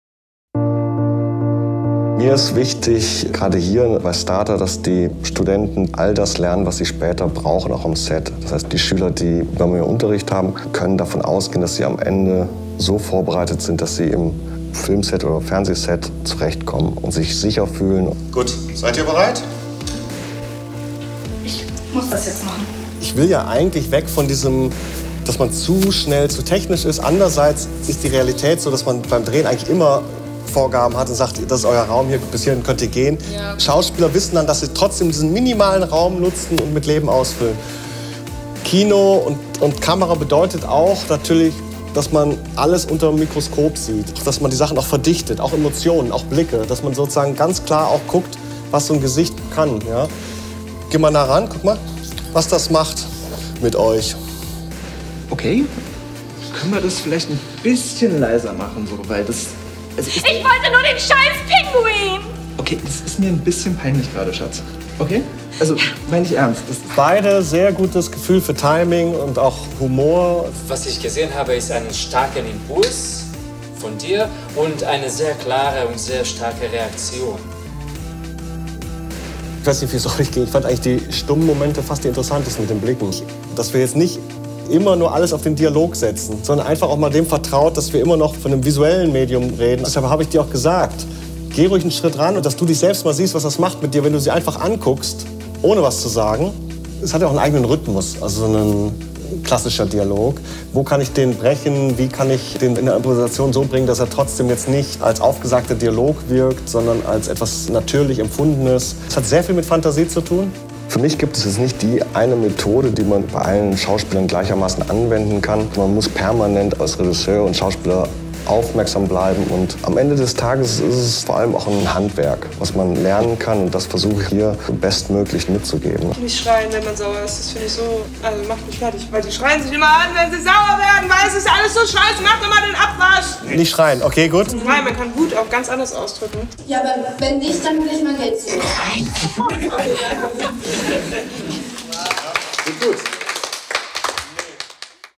STARTER Schauspielschule - Teil 9: Improvisation vor der Kamera
STARTER stellt sich vor! Einblicke aus dem Unterricht an der STARTER, Berliner Schauspielschule für Film und Fernsehen

TEIL 9 - Improvisation vor der Kamera mit STARTER Dozent Nils Dettmann

Einblicke aus dem Unterricht "Improvisation vor der Kamera"

"Beim Drehen hat man immer auch technische Vorgaben. Zum Beispiel "Das ist Euer Raum als SchauspielerInnen, bis hier könnt ihr gehen." Dann muss man  gerade diesen Raum ausnutzen und mit Leben ausfüllen. Kino- und Fernsehproduktionen bedeuten auch Verdichtung von Gefühlen und Blicken.“

Kamera: Nils Dettmann
Schnitt: Soheyla Mohajer
Tonbearbeitung: Steven Hofmann
Projektleitung: Christine Knauff

Fragen zur Schauspielausbildung an unserer Schule oder allgemein zur Schauspielerei?